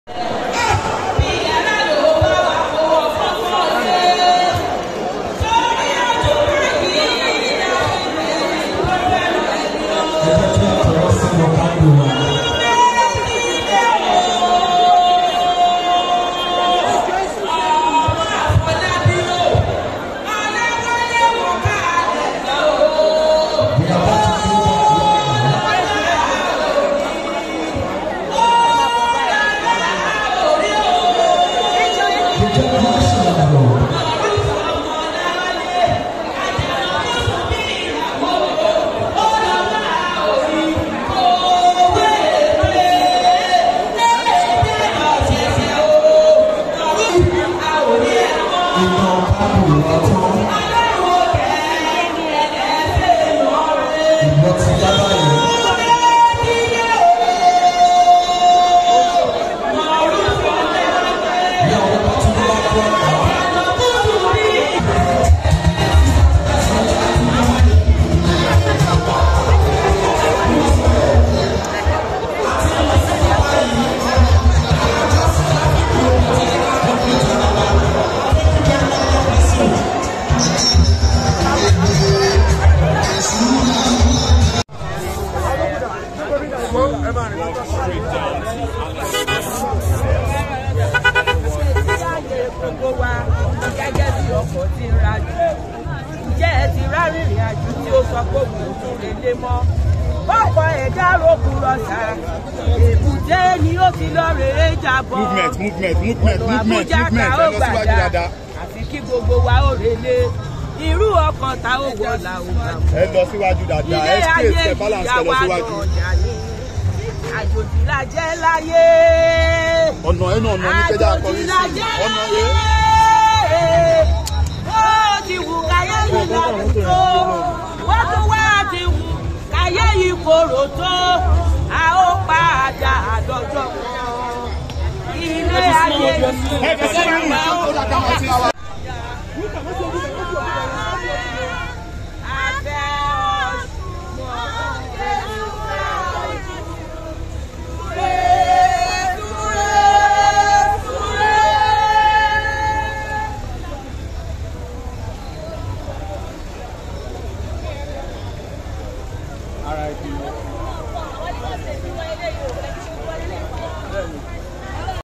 MOMENT ALL ARTISTES CRIES AND LIT CANDLES FOR MURPHY AFOLABI
Murphy Afolabi's Artist Night